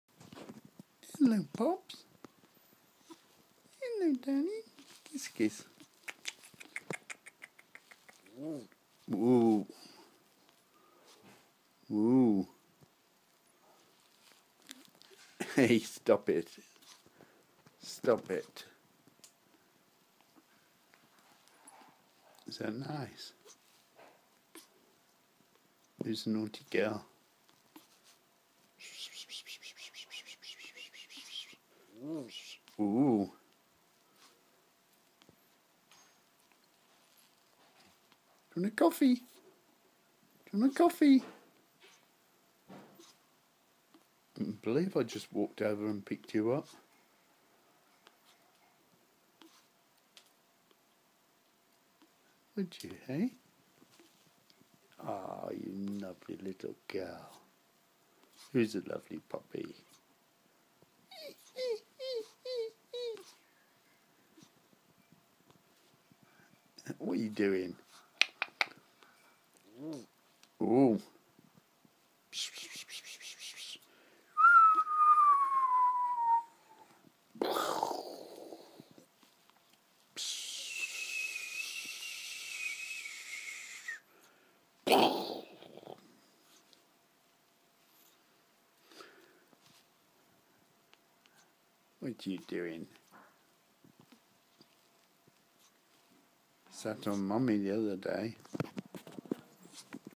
Stroke blog 9th January
Having filled the log baskets  miserable weather I walked over to get Poppy our parrot out who promptly shared my lunch I will carry her back soon W W is going to trim my beard soon as we have special friends coming for dinner